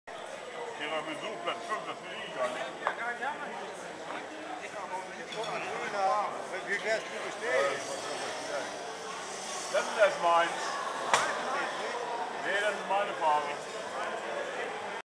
Schwerelos & Zeitlos
Mein Nachtisch kommt ! In der Erlebnisgastronomie "Schwerelos & Zeitlos" in Hamburg.